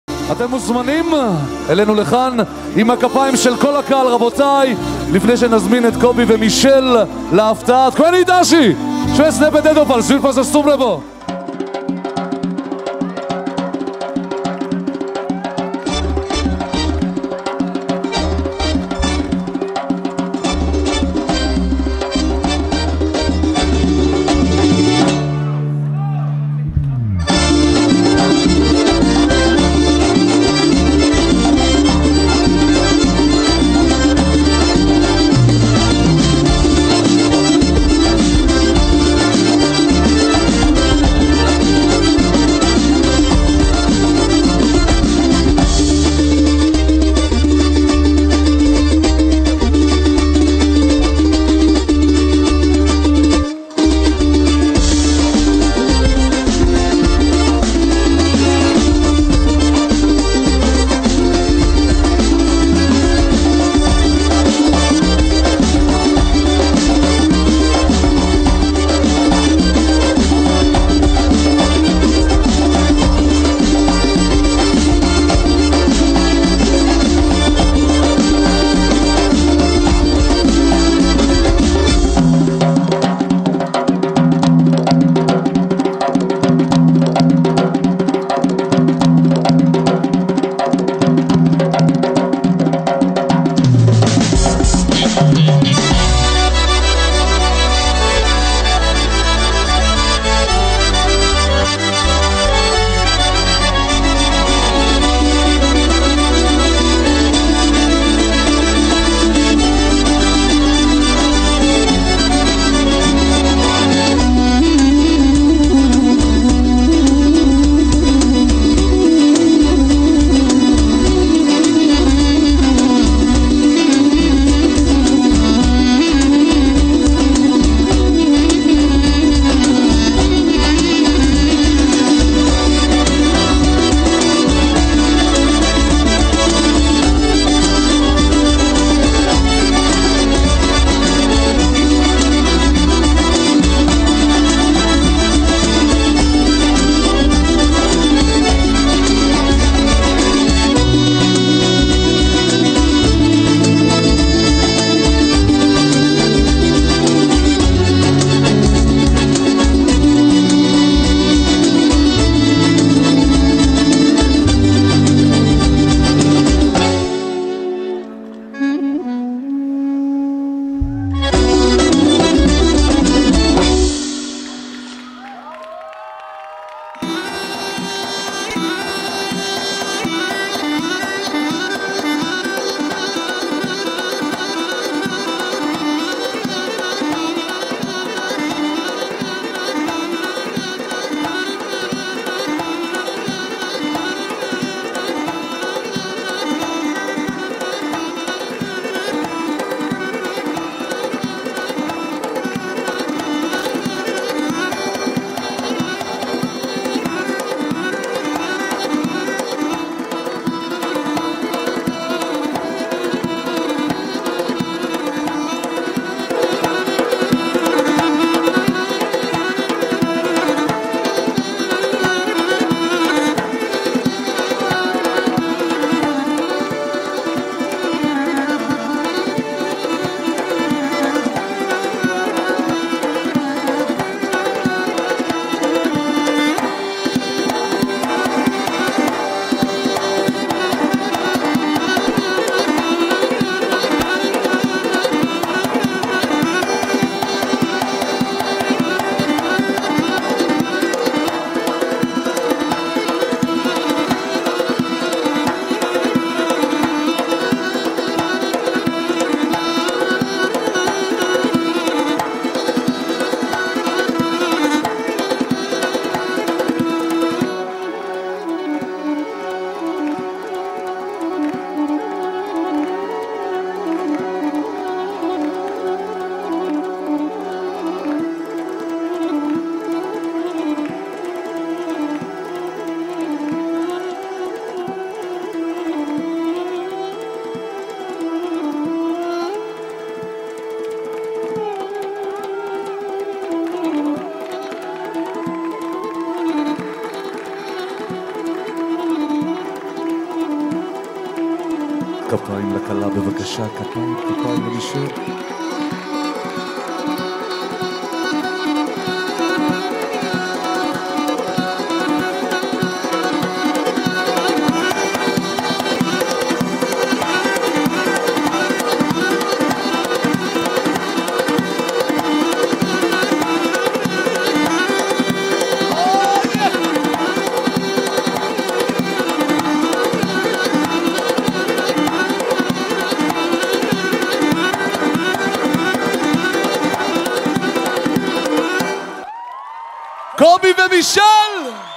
Kartuli georgian wedding dance
Michelle & Kobi are professional Georgian dancers from Israel, this is their traditional Georgian wedding dance.
Choreography: Tamaz Tsitsiashvili from GNB dancegroup Israel
Photography: Yaniv Sofer
Event designer: Simon Hen

მიშელი და კობი ისრაელიდან პროფესიონალი ქართველი მოცეკვავეები არიან, ეს მათი ტრადიციული ქართული საქორწილო ცეკვაა.
ქორეოგრაფია : თამაზ ციციშვილი, GNB- ის საცეკვაო ჯგუფი ისრაელი
ფოტოგრაფია : yaniv sofer
ღონისძიების დიზაინერი : სიმონ ჩენი